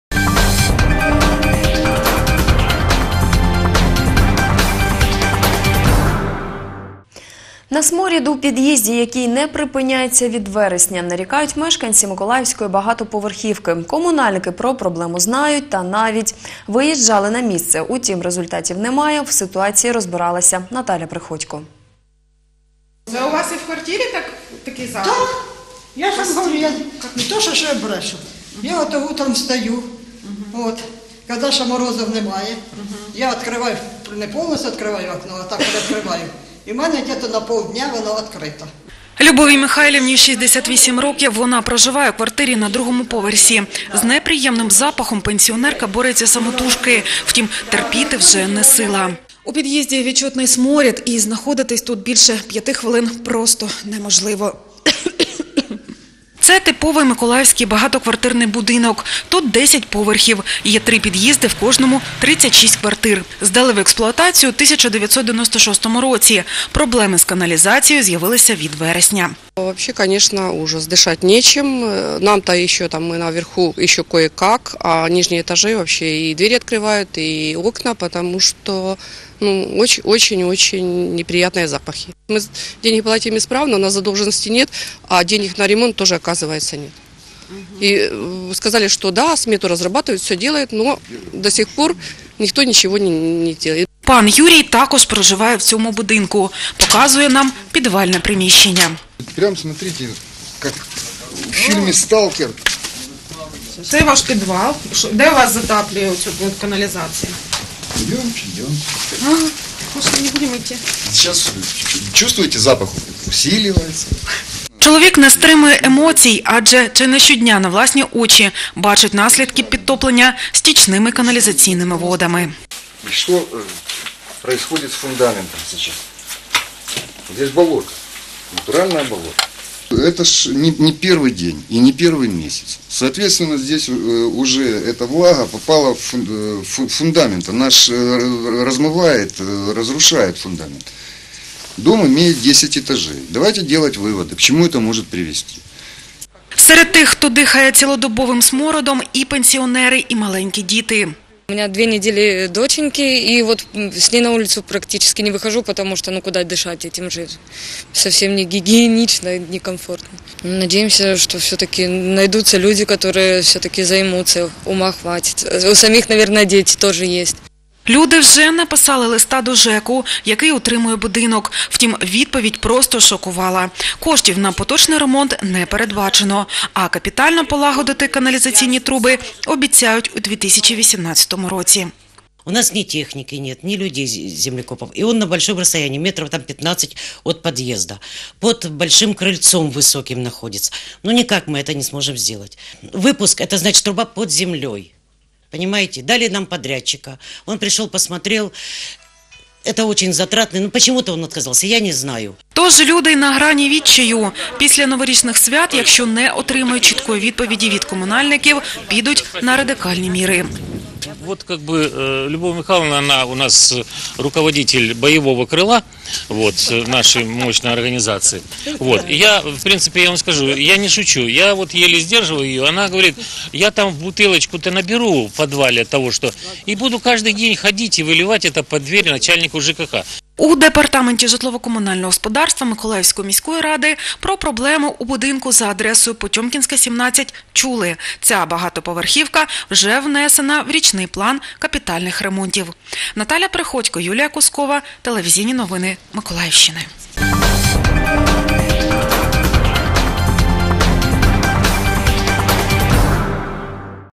ТНМ — сюжет. На сморід у під’їзді нарікають мешканці миколаївської багатоповерхівки
На сморід у під’їзді, який триває від вересня, нарікають мешканці миколаївської багатоповерхівки. Комунальники про проблему знають та навіть виїжджали на місце.  Втім, результатів немає. У ситуації розбиралися наші кореспонденти - Наталя Приходько і Юлія Кускова.